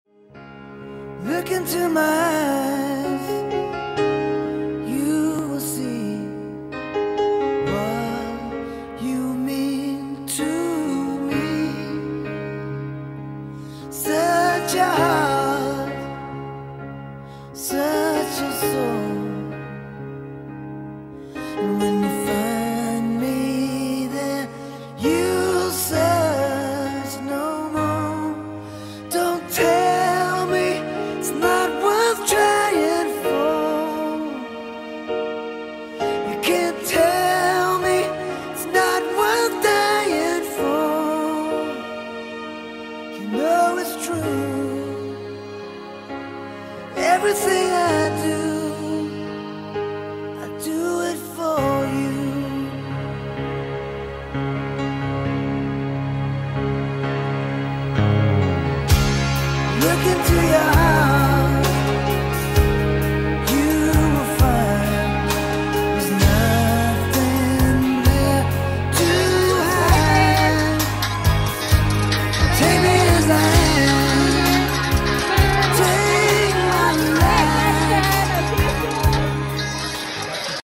LA D2 VIP Hi-touch
LA day 2 VIP - Hi-touch experience with ATEEZ
Love you, miss you!

I am a Dreamer & ATEEZ is my dream